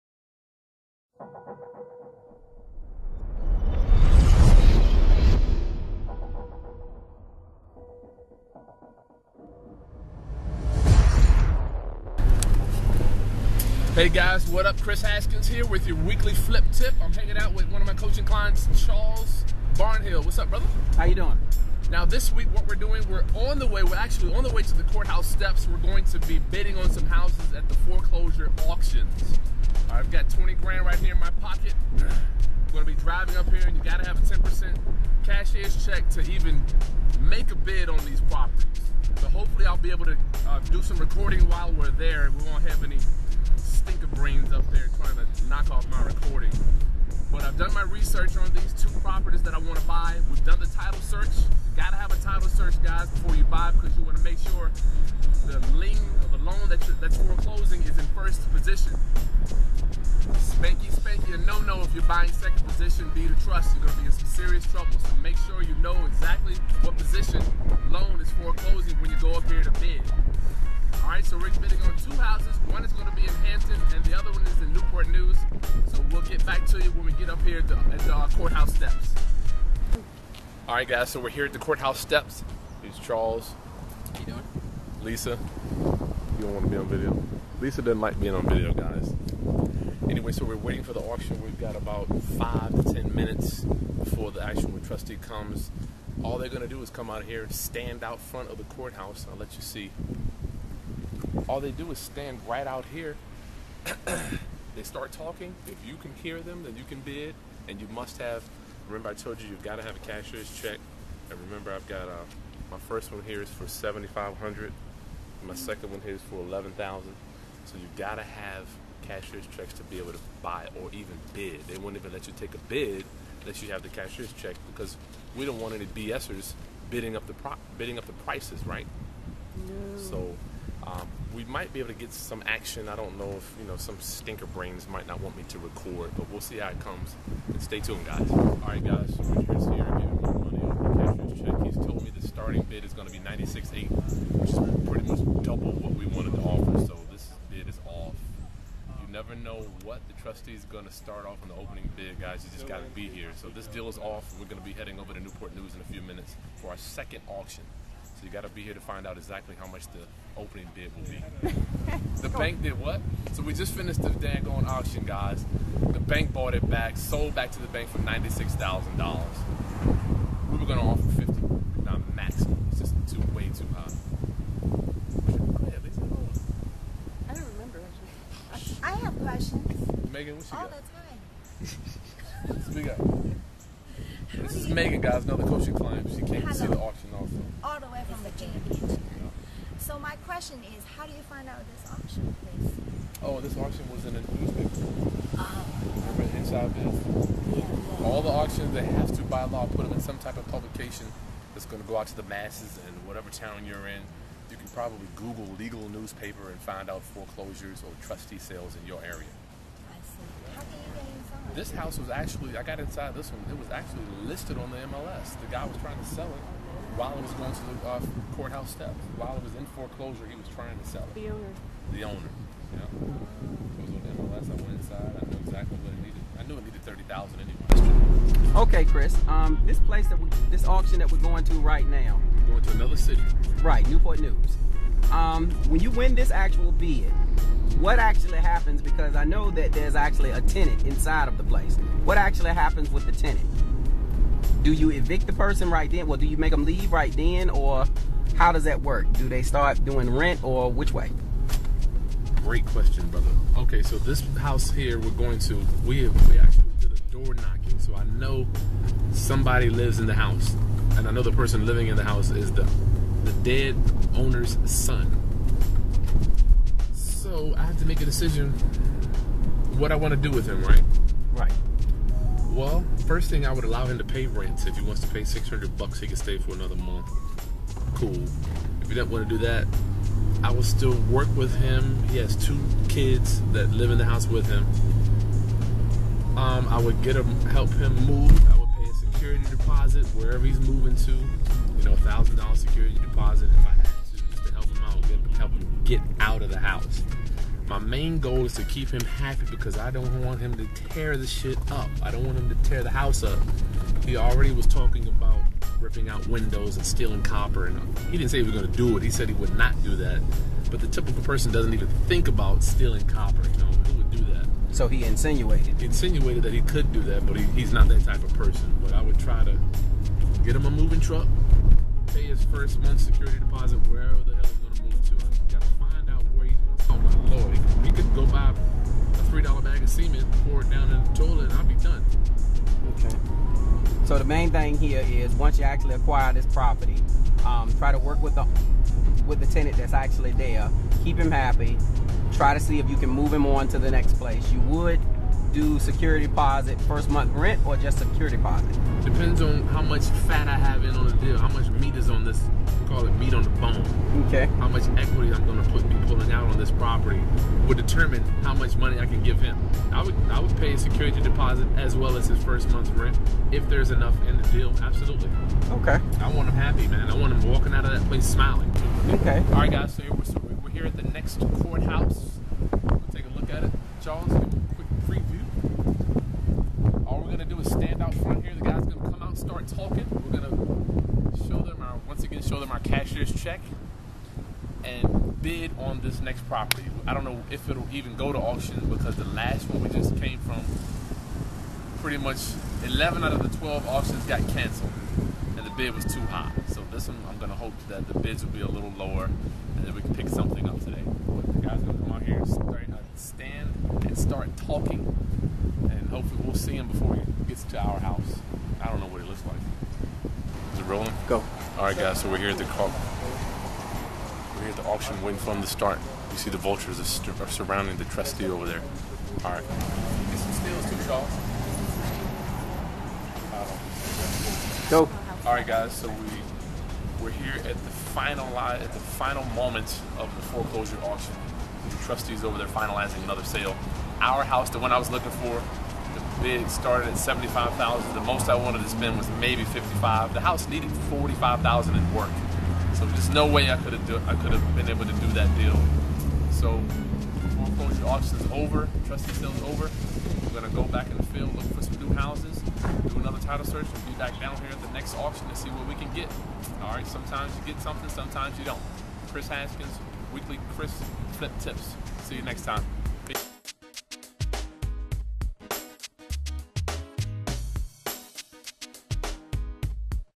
How to buy a house at the foreclosure auction, Part 2
Ride along with kris haskins to see how to buy a foreclosed house at the courthouse steps.  Claim your free copy of my Real Estate Investing Ebook below:
This is my favorite way to buy a house because huge amounts of equity is created in minutes with the foreclosing trustee sale.  The home owner hasn't paid their mortgage and now the home is auctioned off to the highest bidder unless the opening bid is too high, then the bank just takes it back...